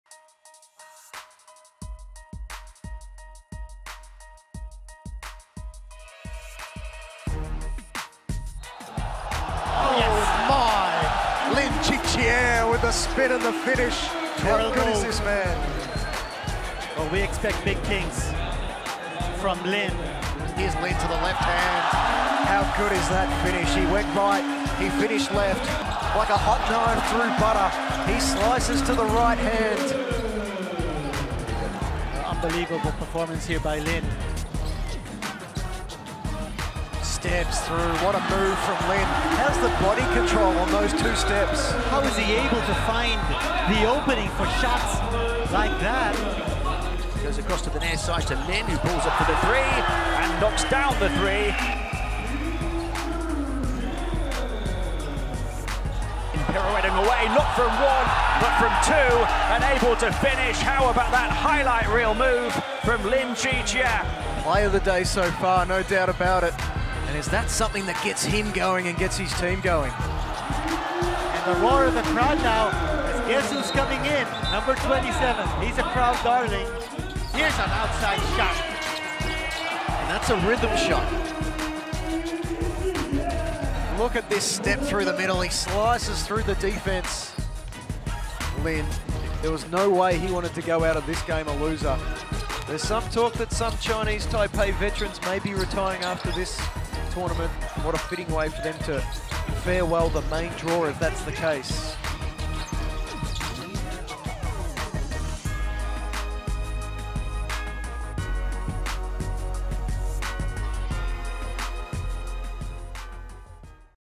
Chih-Chieh Lin: Highlight Reel - 2015 FIBA Asia Championship
Chinese Taipei's Chih-Chieh Lin has been one of the most spectacular and efficient players at this year's FIBA Asia Championship. Check out some of his highlights!